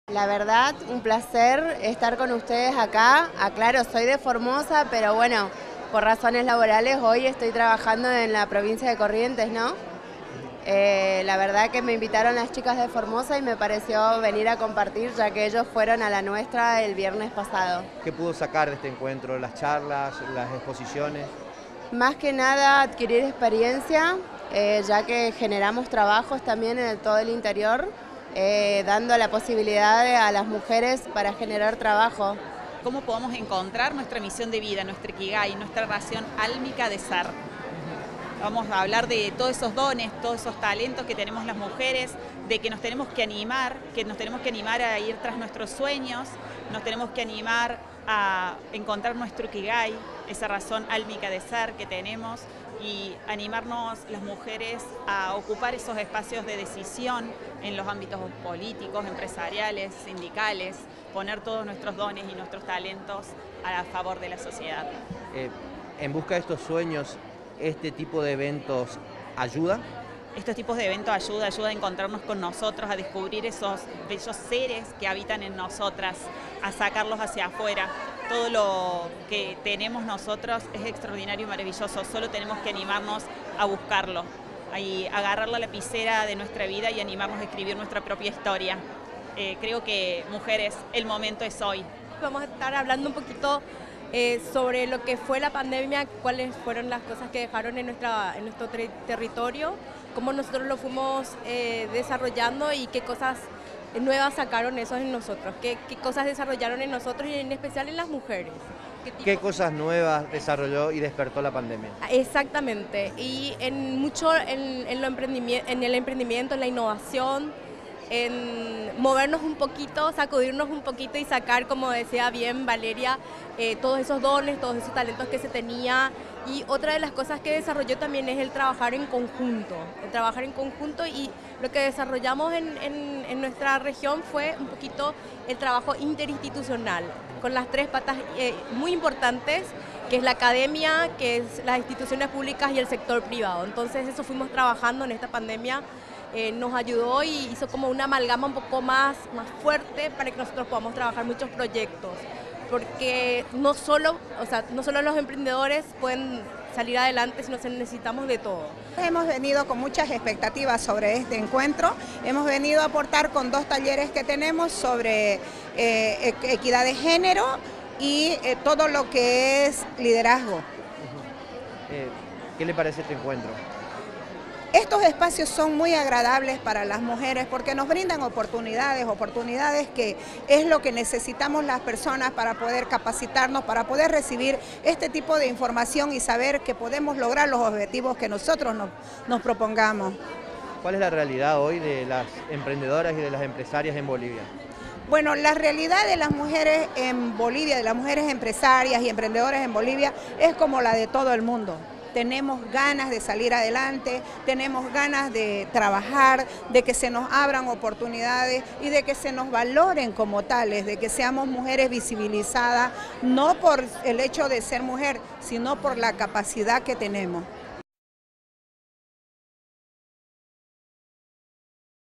MUJERES EMPRENDEDORAS Y EMPRESARIAS FUE EL TEMA DEL CONGRESO ORGANIZADO POR CAMEFOR
SE CONCRETÓ EL SEXTO CONGRESO INTERNACIONAL “MUJERES CON VALOR, DEJANDO HUELLAS EN LA REACTIVACIÓN ECONÓMICA”, ORGANIZADO POR CAMEFOR.
LA JORNADA CONTÓ CON LA DISERTACIÓN SOBRE TEMAS REFERIDOS A LAS MUJERES EMPRENDEDORAS Y EMPRESARIAS, EXPERIENCIAS Y TESTIMONIOS DEL IMPACTO DIRECTO DE LA MUJER EN LA VIDA ECONÓMICA DE SU COMUNIDAD.
LOS PRESENTES TAMBIÉN, DISFRUTARON DE UNA EXPOSICIÓN DE TRABAJOS ARTESANALES, EMPRENDIMIENTOS Y PROYECTOS ENCARADOS POR MUJERES.